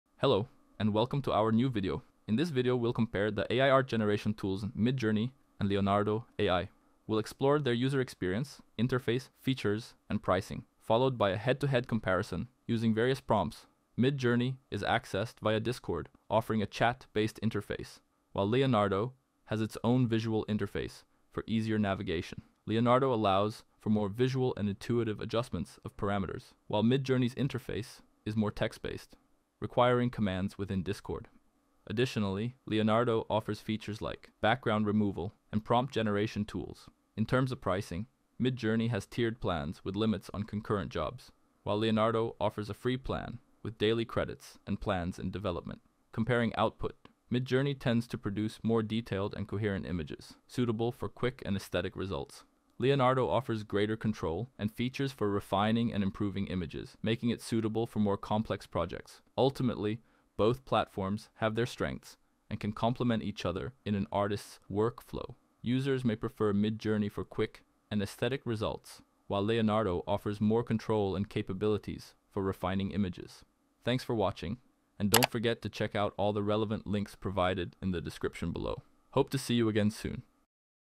Midjourney vs Leonardo AI | Which is better in 2024?
Join us as we compare the two leading AI platforms, Midjourney and Leonardo AI, to determine which is the better option for 2024. We'll analyze their features, performance, and potential future developments to help you make an informed decision. Whether you're a developer, business owner, or AI enthusiast, this video will provide valuable insights into the strengths and weaknesses of these revolutionary technologies.